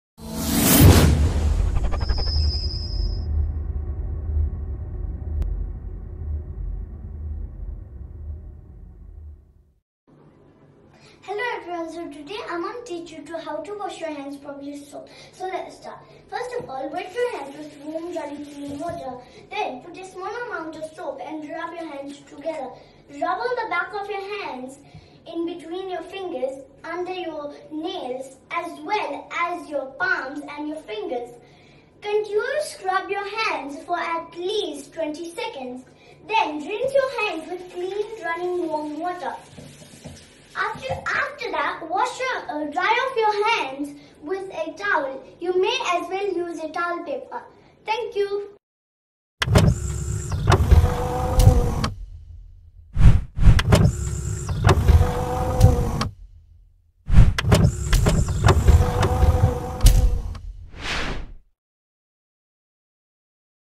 Kids Coronavirus Hand Wash Process--COVID-19
Hand Wash Demonstration for Coronavirus by Kids